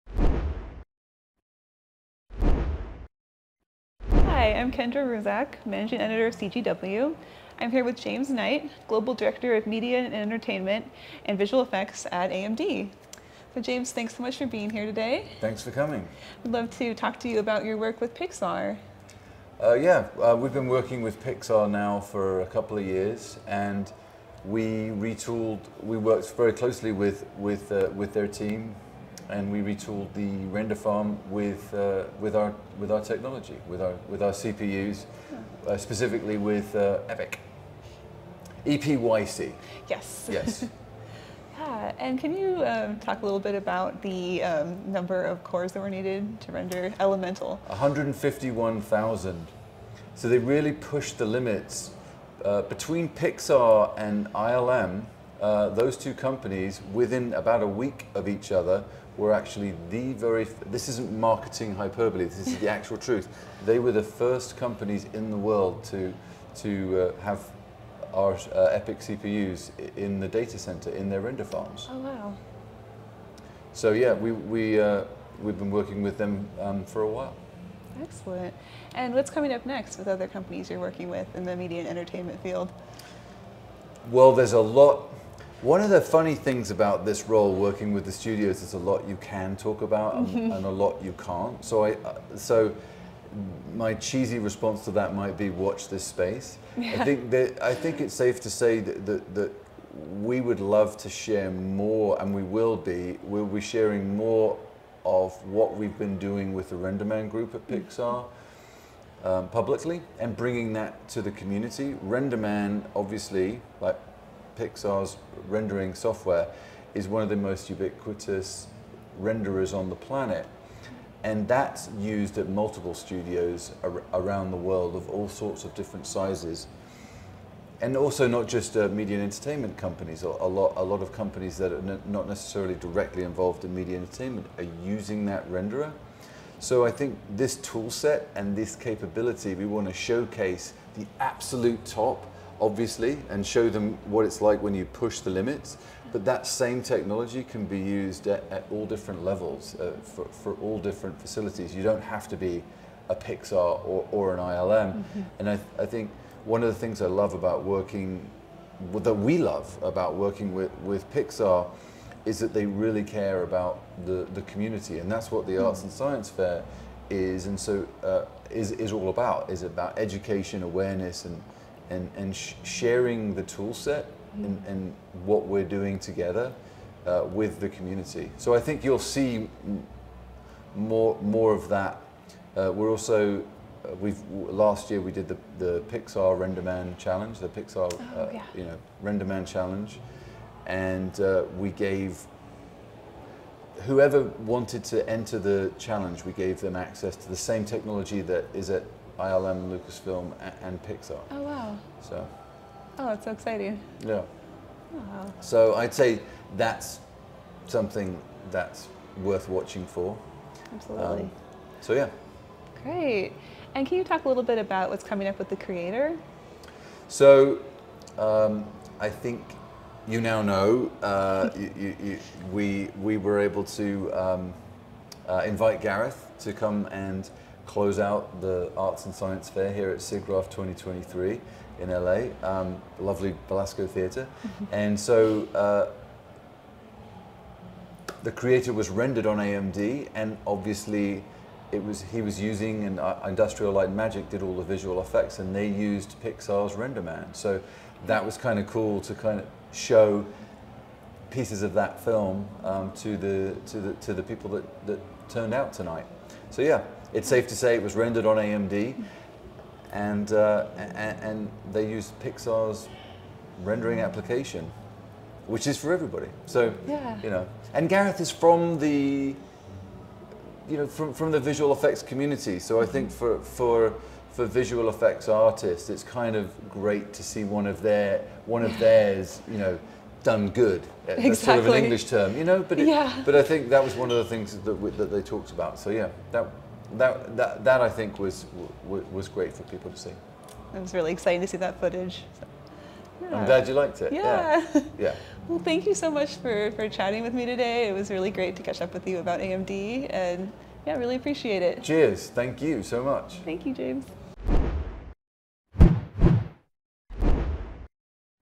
AMD James Knight
CGW's Kendra Kuczak interviews James Knight of AMD talks about The Creator and the massive AMD based renderfarm Pixar used on Elemental, with 151,000 cores.